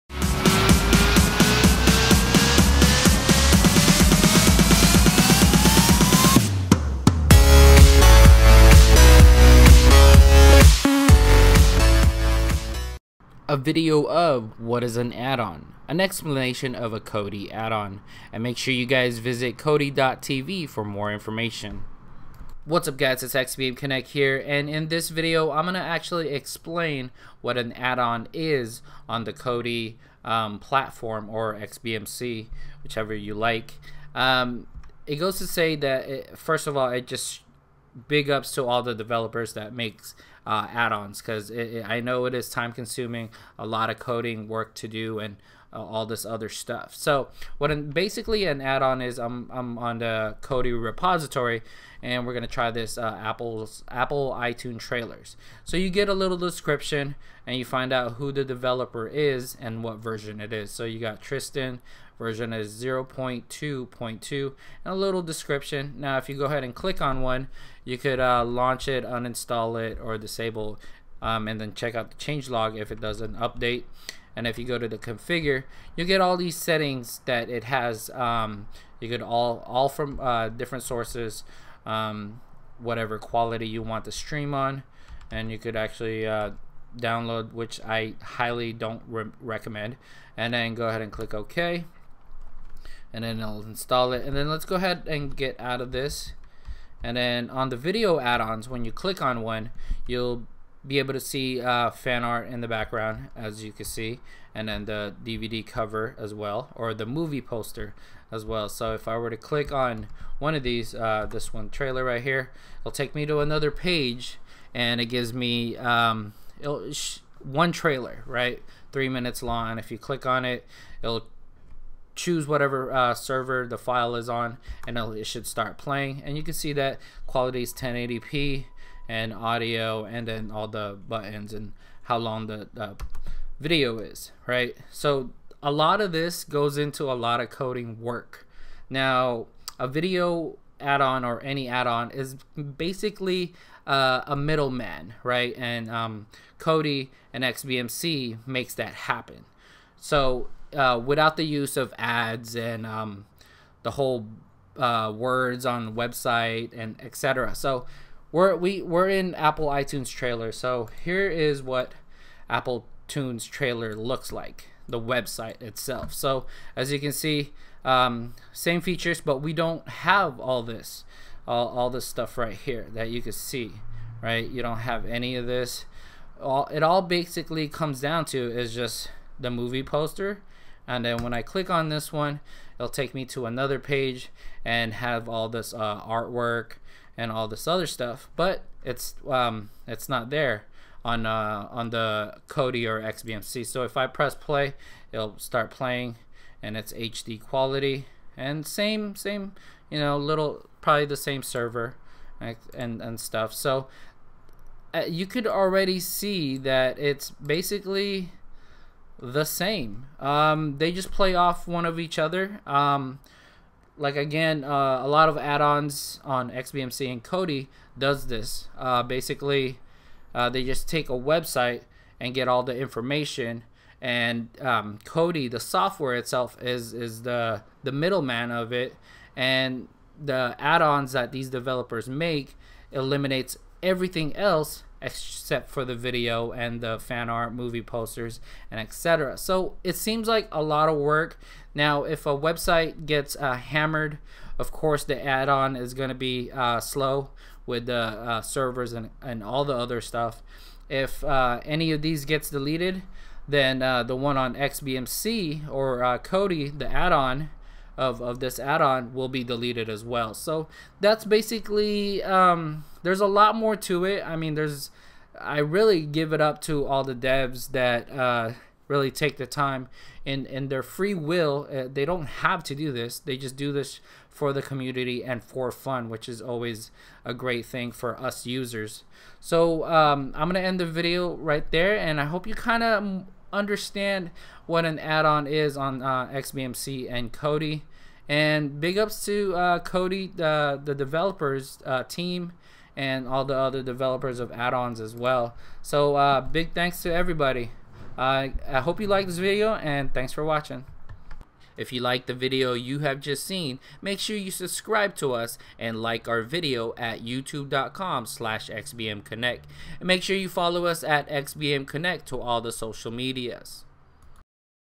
What is a Kodi Add on?
My best explanation of what i think an Add-on does on xbmc/kodi. So please bare with me.